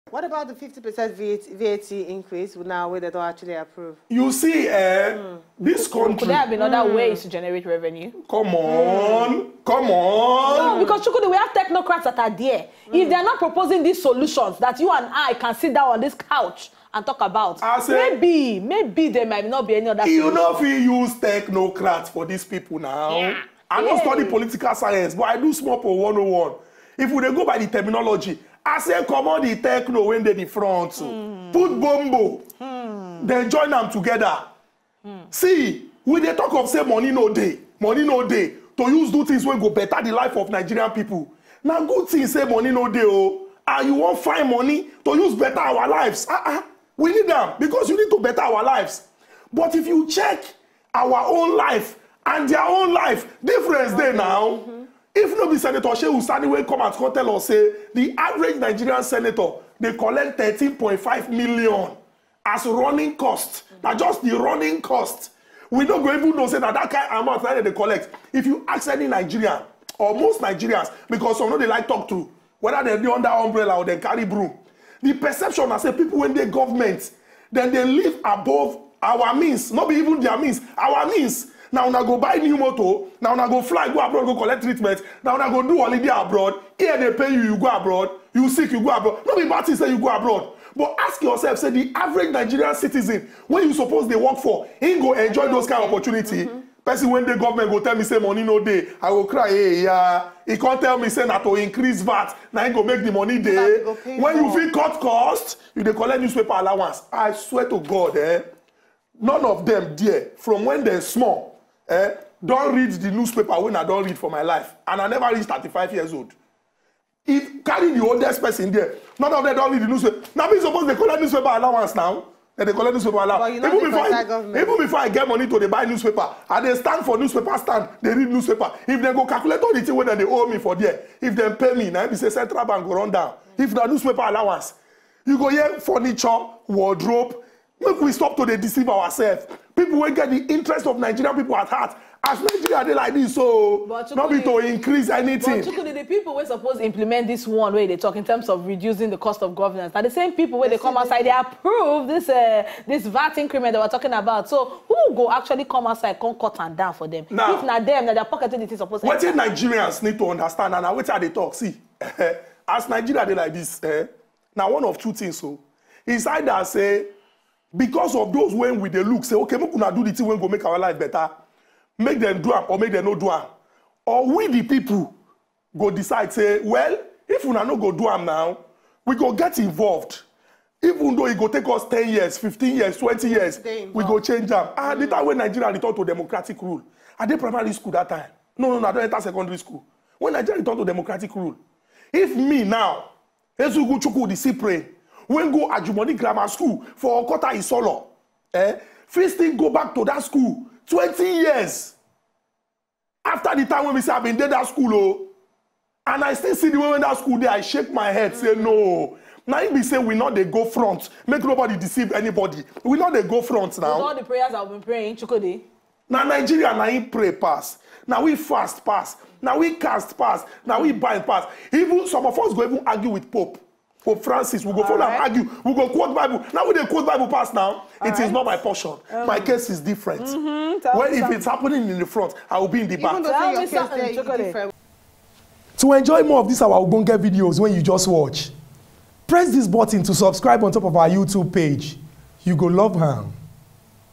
OUTRAGE AS SENATE APPROVES 50% VAT HIKE wazobiamax
The Federal Executive Council had in September this year approved an increase in VAT and sent a bill to the National Assembly for consideration. President Muhammadu Buhari later on October 14 sent the bill to the legislature.

Follow WazobiaMax TV on Twitter value added tax vat online vat explained